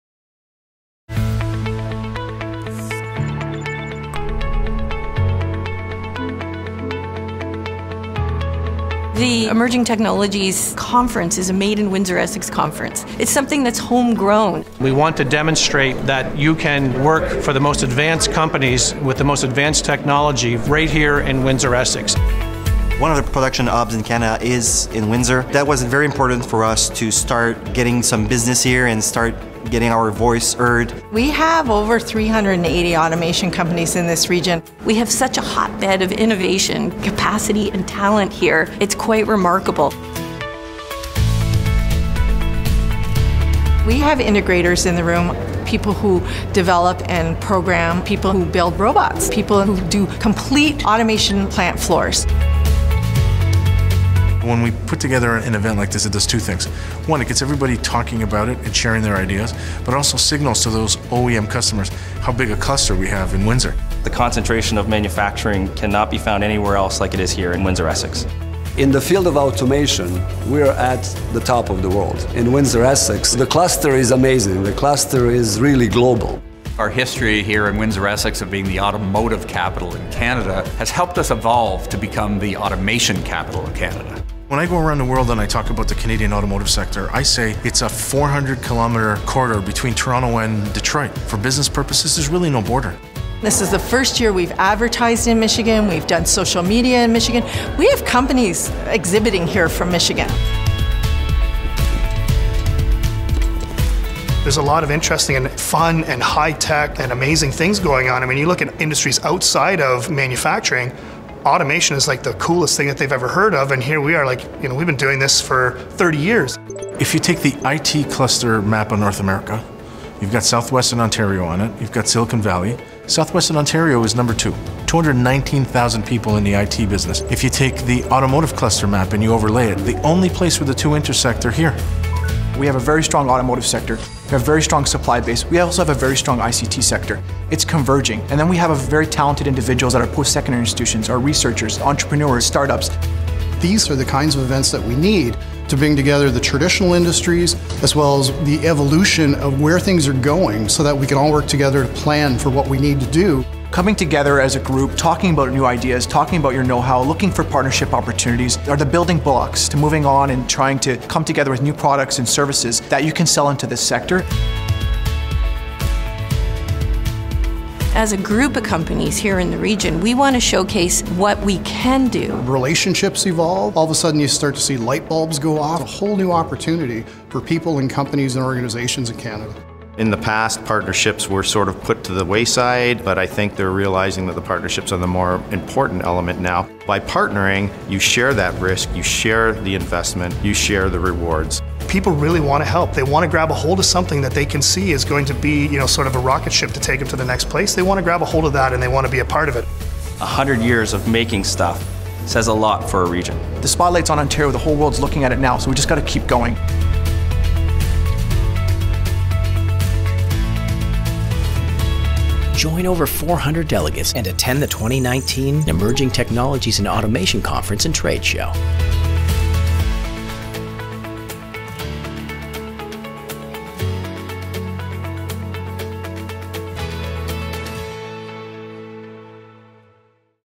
2018 Emerging Technologies in Automation Conference and Trade Show
The keynote address “On the Road to Autonomous Cars - Trends and Technologies was presented by Grant Courville, VP, Product Management and Strategy with Blackberry’s QNX Software Systems.

Attendees heard presentations from many experts in automation technology such as: Alexandre Valliere with Alworx, Chris Blanchette with FANUC America Corporation and complimented with a full day of outstanding presenters and exhibitors showcasing the latest developments in automation at the interactive trade show.

SAVE THE DATE! Tuesday, September 10, 2019 and attend the event!